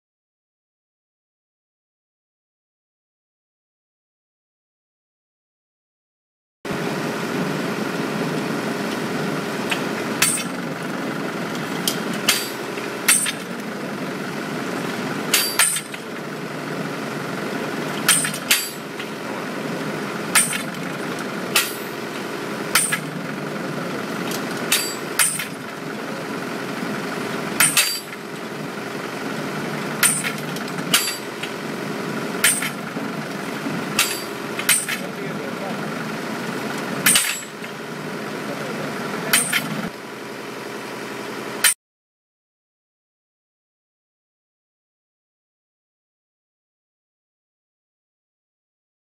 Standard penetration test Automatic vs. Manual Hammer
Standard penetration test - Automatic vs. manual procedure